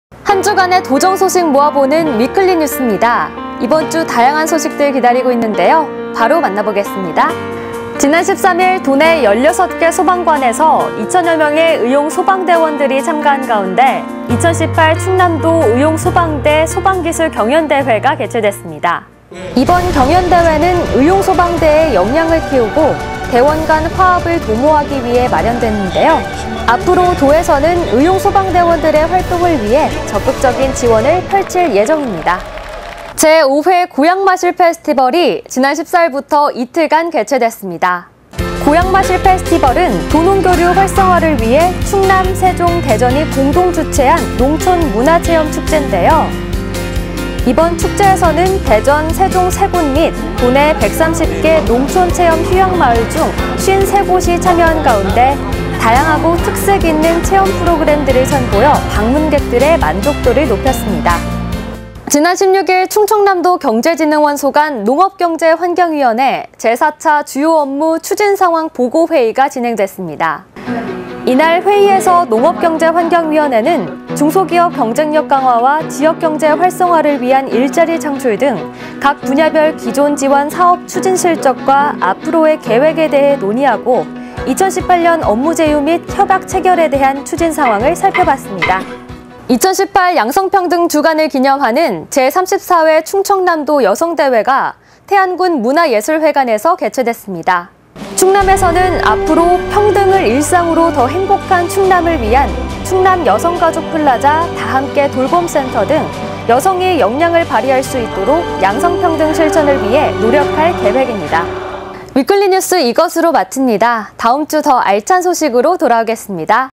7월 3주차 충청남도 위클리 뉴스
1. 의용소방대소방기술경연대회
2. 고향마실페스티벌
3. 도의회농경환위
4. 제34회충청남도여성대회